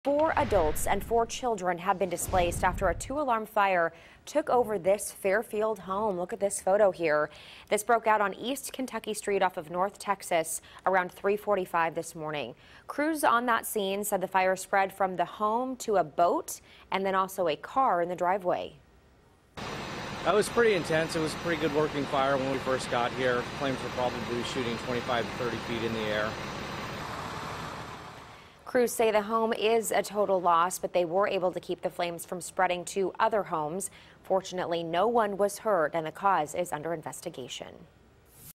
8 People Displaced After 2-Alarm Fire Spreads From Boat To Fairfield Home
Authorities say no one was hurt after a fire consumed a boat and vehicle parked at a Fairfield home early Wednesday morning.